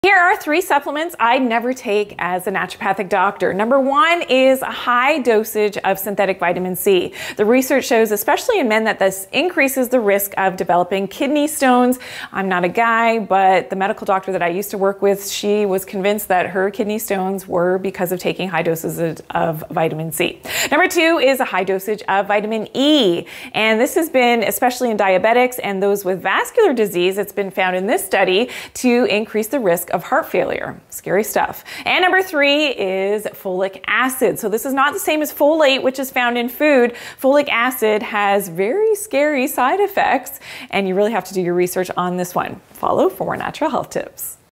3 Supplements I Would NEVER Take as a ND
3 Supplements I Would NEVER Take as an ND

Dr. Janine shares three supplements she would NEVER take as a Naturopathic Doctor.  The first is high doses of synthetic vitamin C.  This has been shown to increase the risk of kidney stones, especially in men.  The second is high doses of vitamin E.  Dr. Janine explains how high doses of vitamin E have been shown in research to cause a higher risk of heart failure, especially in diabetics and people with vascular diseases.  Lastly, Dr. Janine explains why she would NEVER take folic acid as a Naturopathic Doctor.  She talks about how this is NOT the same as folate, which is naturally found in foods and has scary side effects.

Links to studies mentioned in this short
Ascorbic Acid Supplements and Kidney Stone Incidence Among Men: A Prospective Study
Effects of long-term vitamin E supplementation on cardiovascular events and cancer: a randomized controlled trial

J9CON230184